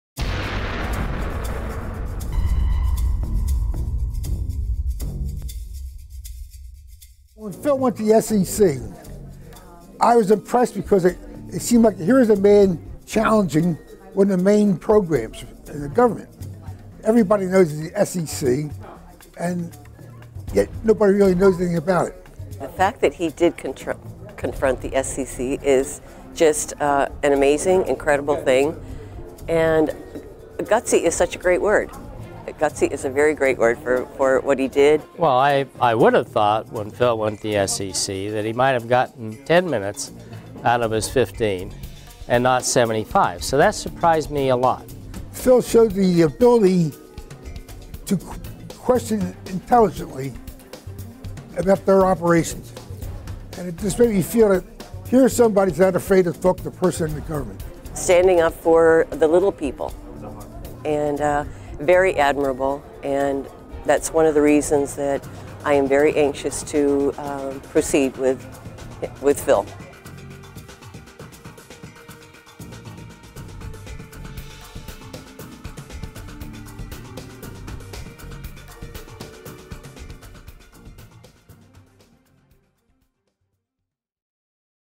Consumers react to Phil Cannella's interview with H. David Kotz - Spring Mill Manor
At Phil Cannella's Crash Proof Retirement Presentation, consumers react to Phil Cannella's interview with the Inspector General of the SEC, H. David Kotz.

"When Phil went to SEC, I was impressed because it seemed like here is a man challenging one of the main programs and the government. Everybody knows the SEC and yet nobody really knows anything about it."

"The fact that he did confront the SEC is just an amazing incredible thing and gutsy is such a great word it got see it's a very great word for what he did."

"Well I I would have thought when Phil went the SEC that he might have gotten ten minutes out of his 15 and not 75 so that surprised me a lot."

"Phil showed the ability to question intelligently about their operations and just made you feel it here's somebody not afraid to talk to the person in the government. Standing up for the little people and very admirable and that's one of the reasons that I am very anxious to proceed with with Phil."